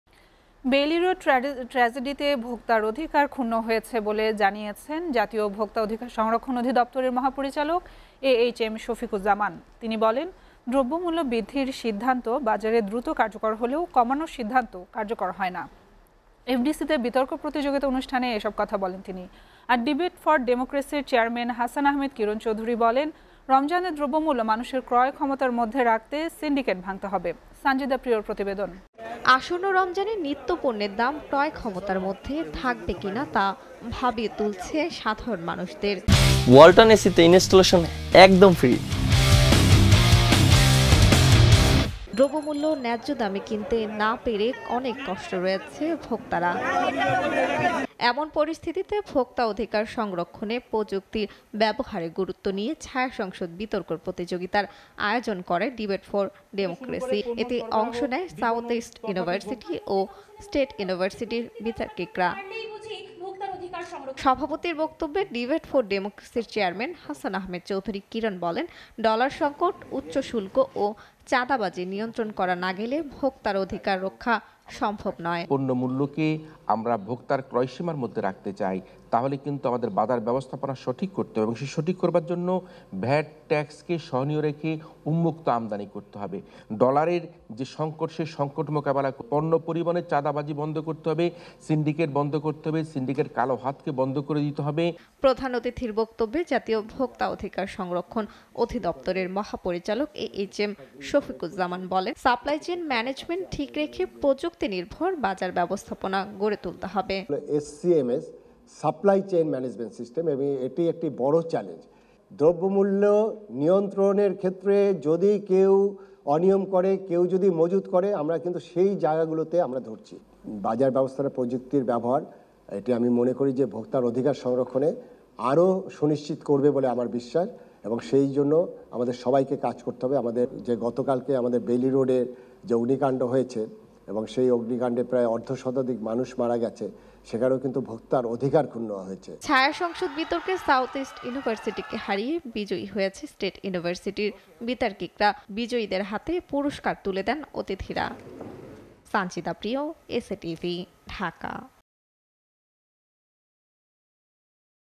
বেইলি রোড ট্র্যা'জেডিতে ভোক্তার অধিকার ক্ষুণ্ন হয়েছে : এ.এইচ.এম সফিকুজ্জামান | SATV NEWS
বেইলি রোড ট্র্যাজেডিতে ভোক্তার অধিকার ক্ষুণ্ন হয়েছে : এ.এইচ.এম সফিকুজ্জামান

SATV Location:
Bangladesh